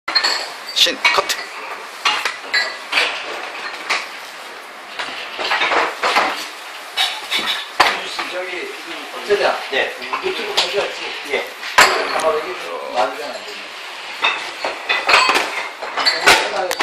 [신기신기] 한정식집 한상 차림 간편 버전
송파구에 있는 이천쌀밥 한정식집 한상 간편 차림

아이디어 GOOD!